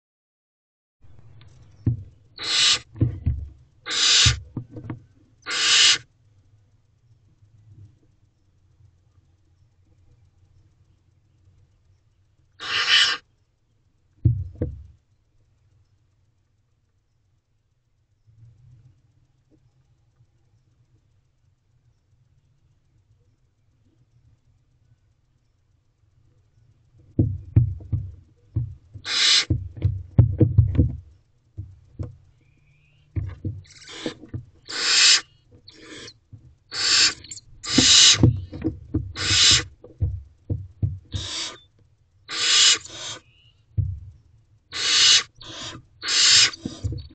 OwletFinley and Charli Are about the Same Size. Owl Box C - 5/4/23 - 7:56 pm
-   Charli and Finley are standing.  Paulie is in the corner Snuggling with Min

Owlets

Owl Boxes